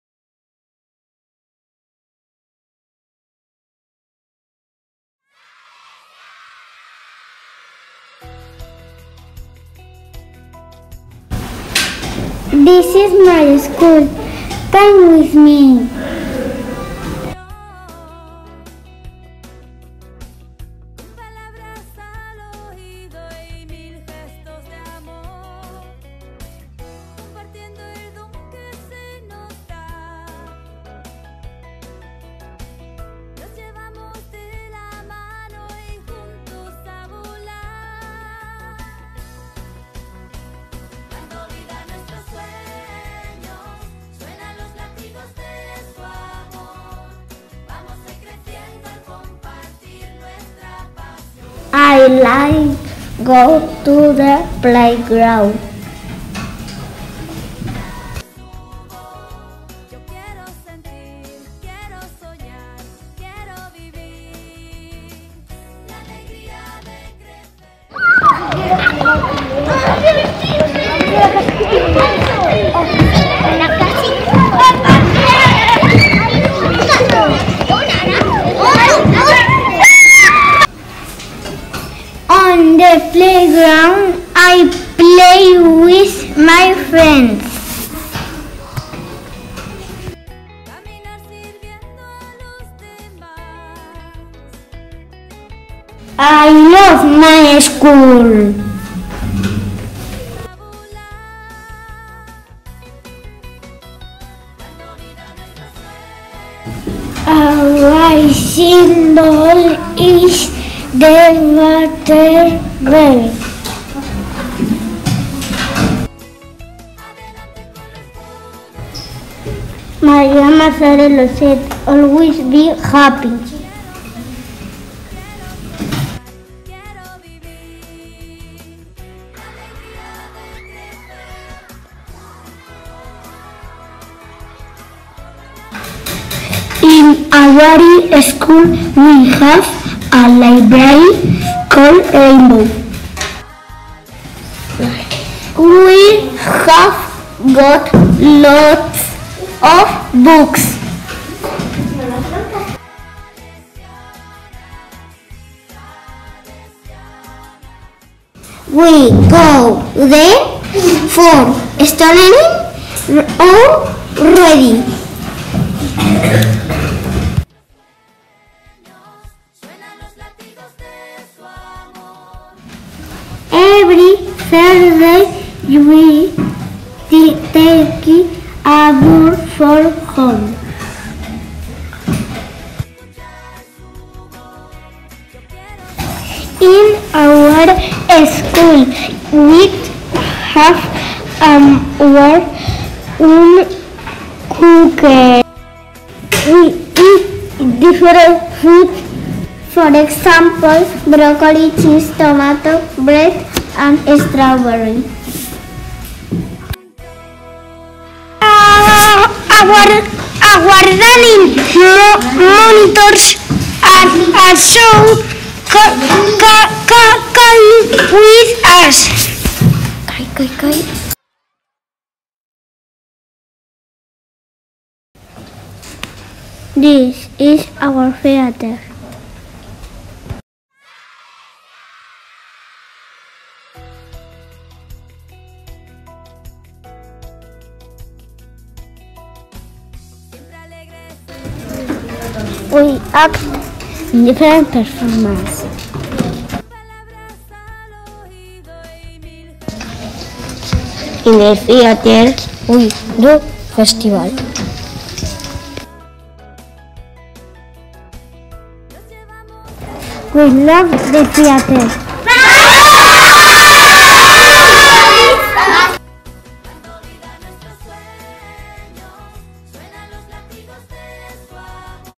PROYECTO INGLÉS PRIMERO DE PRIMARIA
Enseñamos nuestro colegio a otros colegios de Salesianas en un proyecto bilingüe.